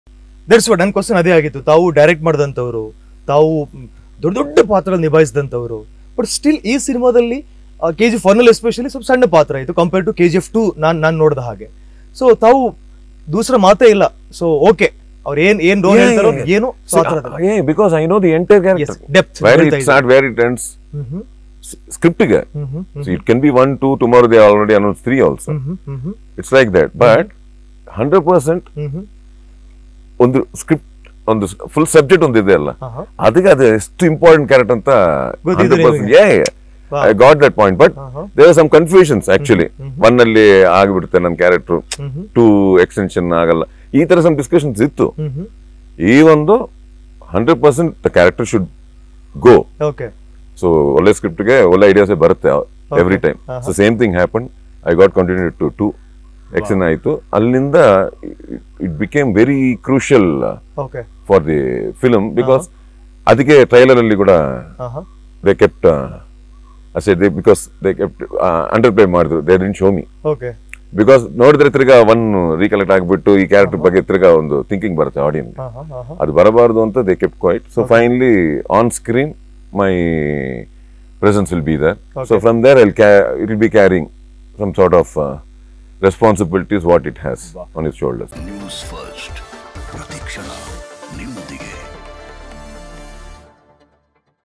Ayyappa P Sharma : KGF-2 ಟ್ರೇಲರ್​ನಲ್ಲಿ ನನ್ನ ತೋರಿಸಿಯೇ ಇಲ್ಲ | Rocking Star Yash | KGF Chapter 2
Ayyappa P Sharma : KGF-2 ಟ್ರೇಲರ್​ನಲ್ಲಿ ನನ್ನ ತೋರಿಸಿಯೇ ಇಲ್ಲ | Rocking Star Yash | Prashanth Neel | KGF Chapter 2 | NewsFirst Kannada